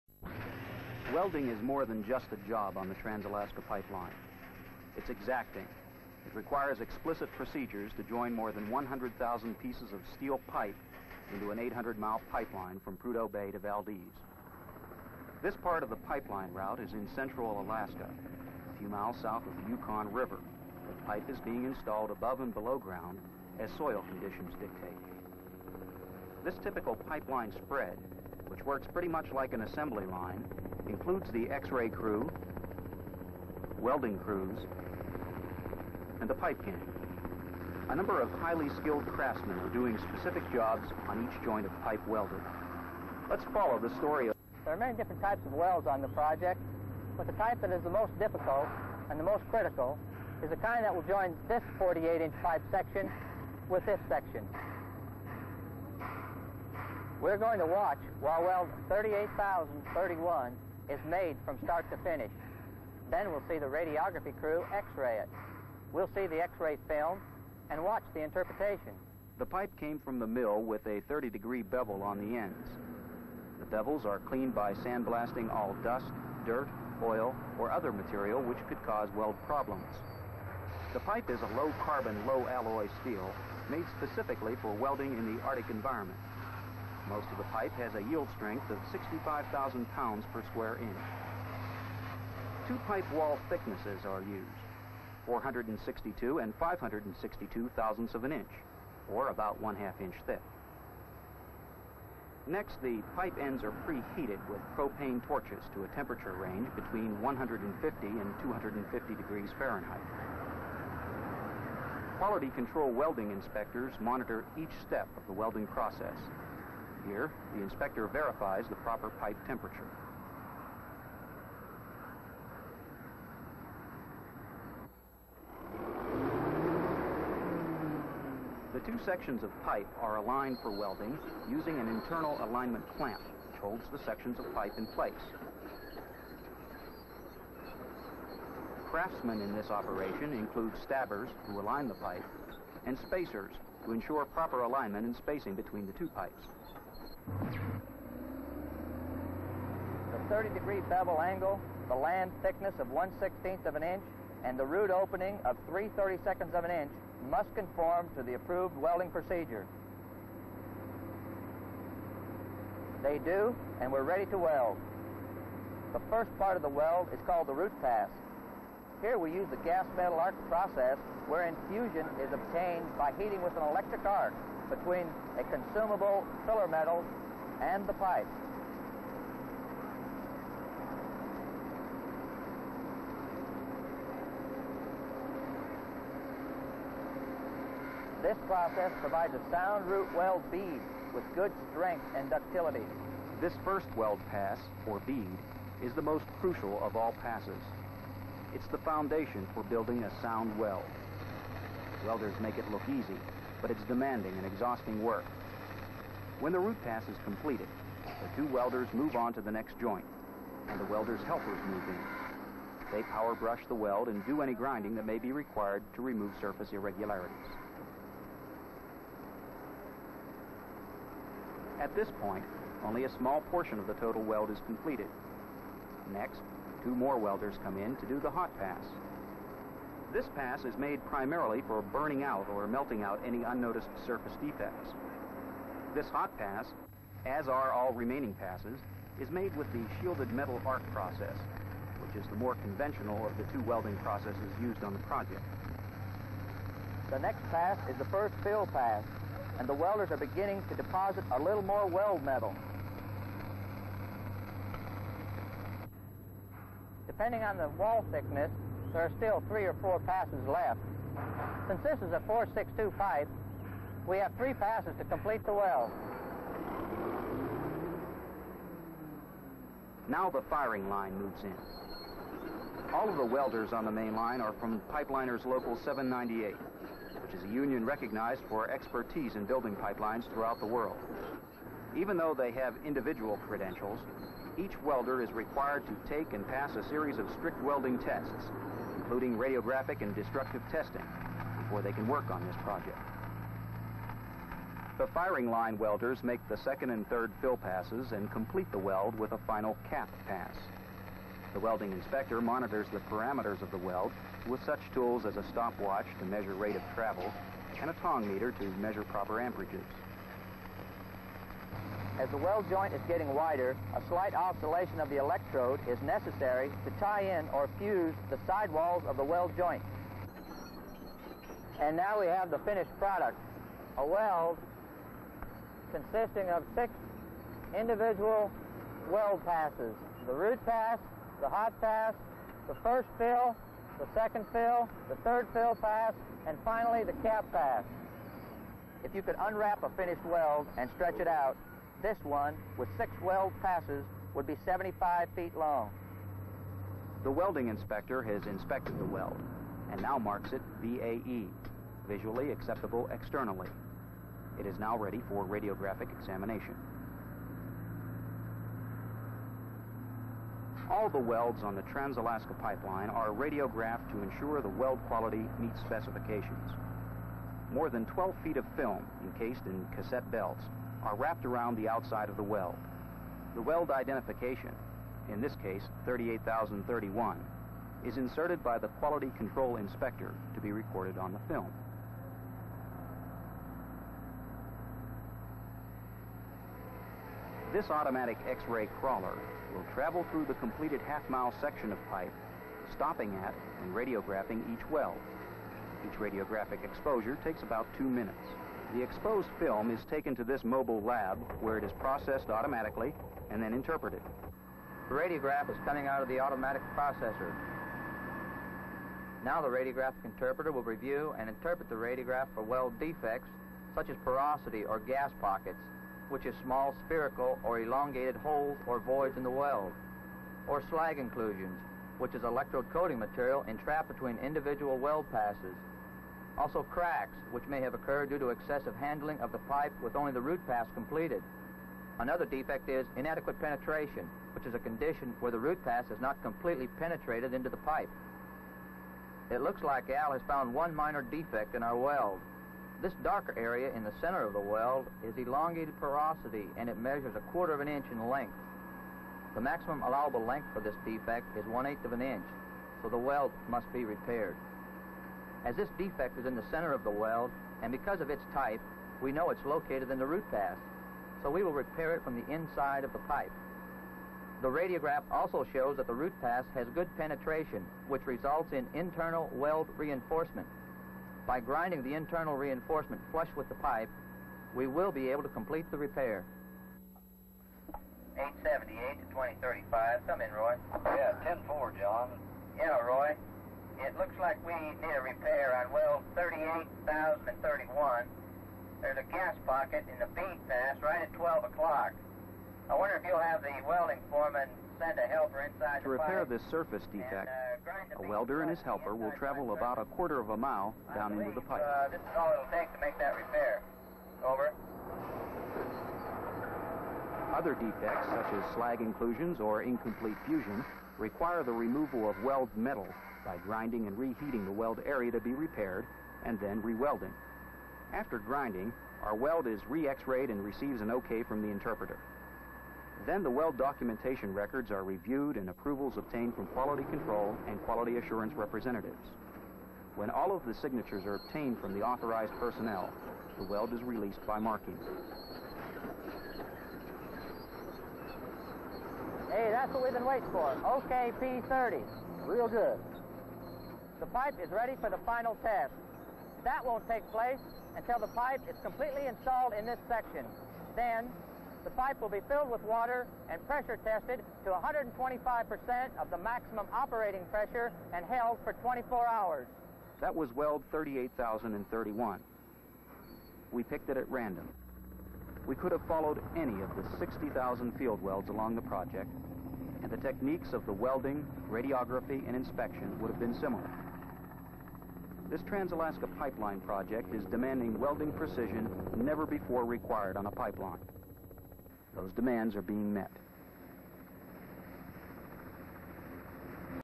Trans Alaskan pipeline welding, 1970's -- Film 9610
Trans Alaskan pipeline welding.